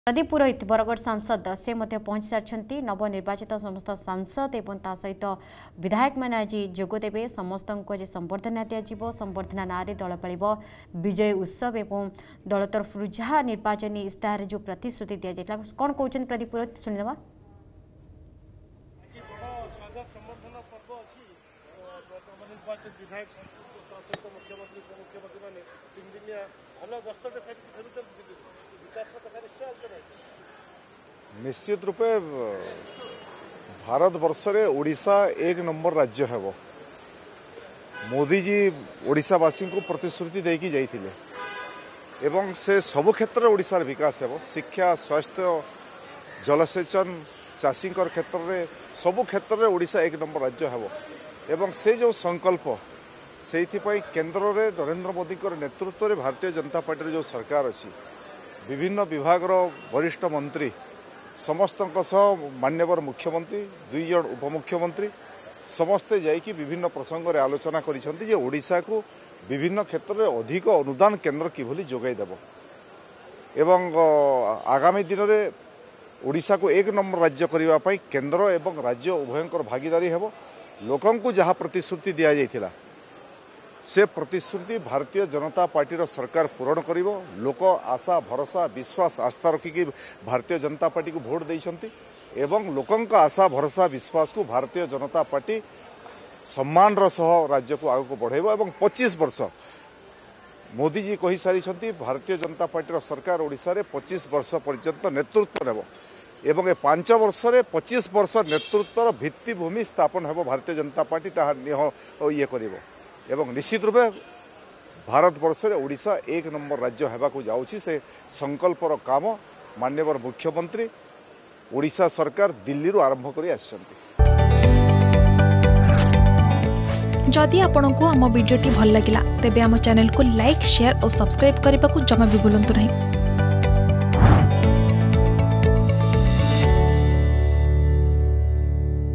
ଆଗାମି ୫ ବର୍ଷରେ ୨୫ ବର୍ଷର ଭିତିଭୂମି ସ୍ଥାପନ ହେବ || Pradeep Purohit
ବିଜେପିର ବିଜୟ ଉତ୍ସବରେ ସାମିଲ ହେବା ପାଇଁ ଭୁବନେଶ୍ବର ବିମାନବନ୍ଦରରେ ପହଞ୍ଚିଲେ ବରଗଡ ସାଂସଦ ପ୍ରଦୀପ ପୁରୋହିତ । ଭୁବନେଶ୍ବର ପ୍ରଦର୍ଶନୀ ପଡ଼ିଆରେ ନବନିର୍ବାଚିତ ସାଂସଦ ଓ ବିଧାୟକଙ୍କୁ ସମ୍ବର୍ଦ୍ଧନା କାର୍ଯ୍ୟକ୍ରମ ।